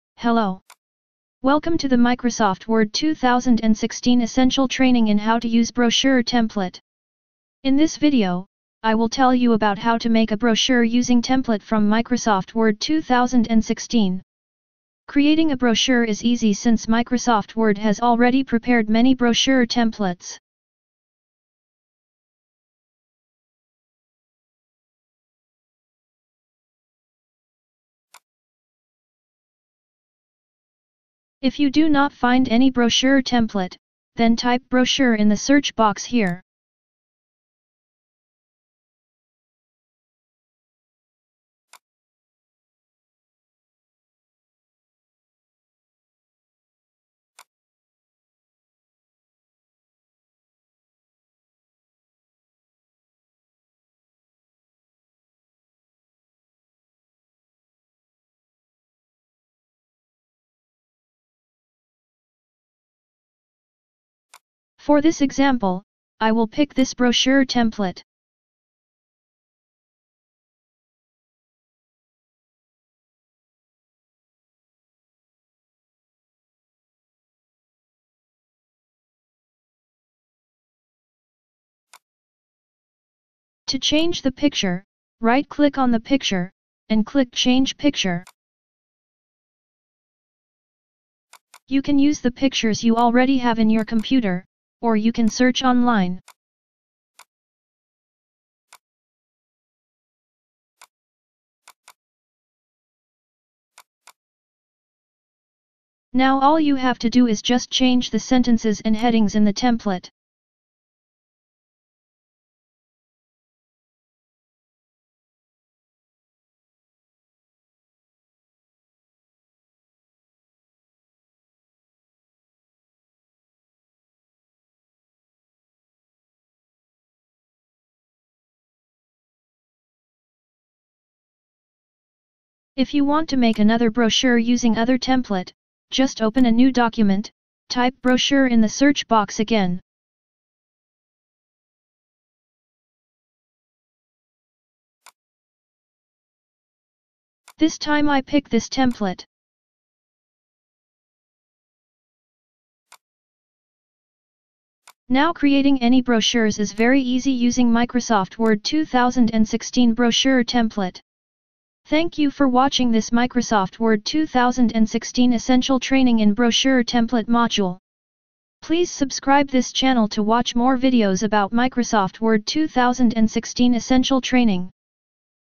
Microsoft Word 2016 Essential Training | Brochure Template
For most of us, making a brochure can be a little time consuming.

But worry no more, we have a Microsoft Word 2016 here. This awesome program is a great helper.

Ms Word 2016 has already prepared many templates you can use anytime you like.

Using those great brochure templates, creating a brochure is just one click away.

Watch this video about how to make a brochure. The method is simple, the video is very short, you will immediately be an expert in brochure.